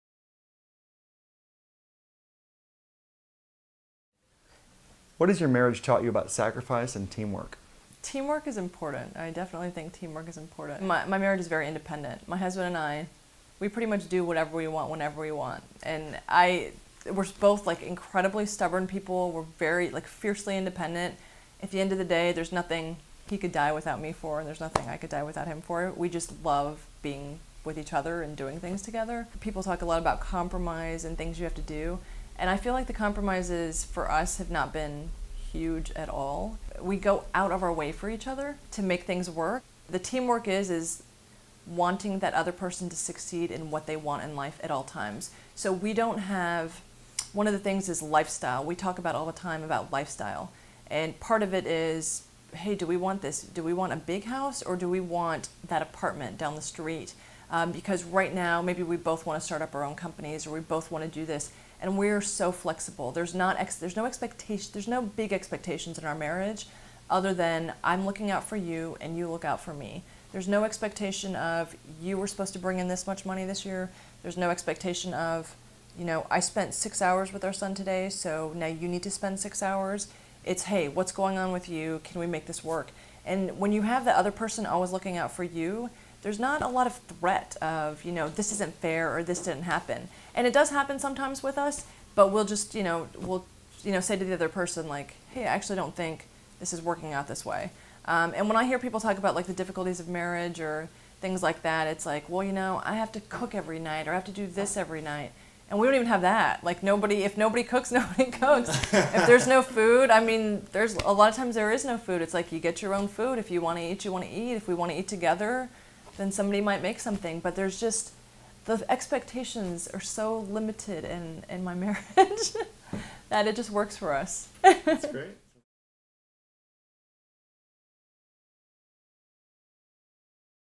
Nina Godiwalla on How to Set Expectations in a Marriage
In Chapter 22 of 22 in her 2011 Capture Your Flag interview, author Nina Godiwalla answers "What Has Your Marriage Taught You About Sacrifice and Teamwork?" She shares how she and her husband blend fiercely independent attitudes with a deep support for one another. Godiwalla is the author of "Suits: A Woman on Wall Street". She is also a public speaker on workplace diversity and founder and CEO of Mindworks, where she teaches mind-based stress reduction techniques to help organizations improve employee wellbeing. Godiwalla holds an MBA from the Wharton School of Business, an MA in Creative Writing from Dartmouth University and her BBA from the University of Texas at Austin.